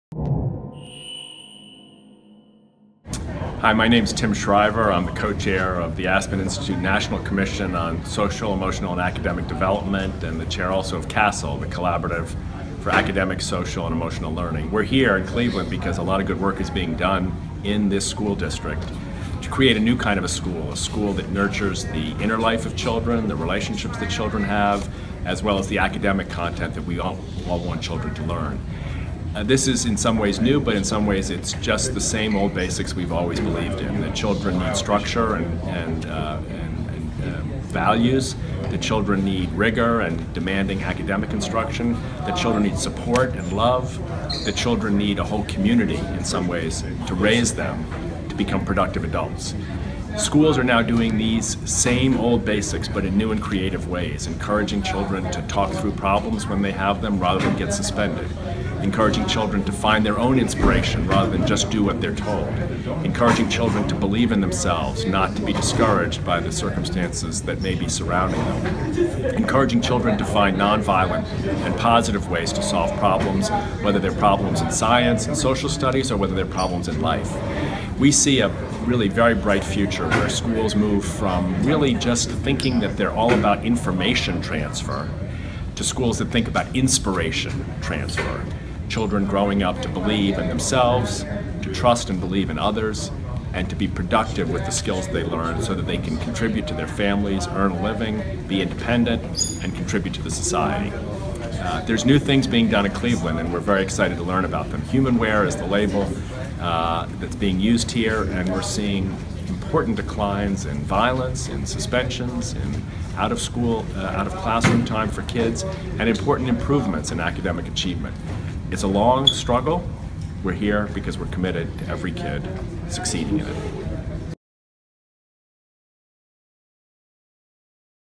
Social and emotional learning Co-chair Tim Shriver
Tim Shriver, a co-chair of the Aspen Institute commission on social and emotional learning, talks about the importance of the field and why the commission is visiting Cleveland.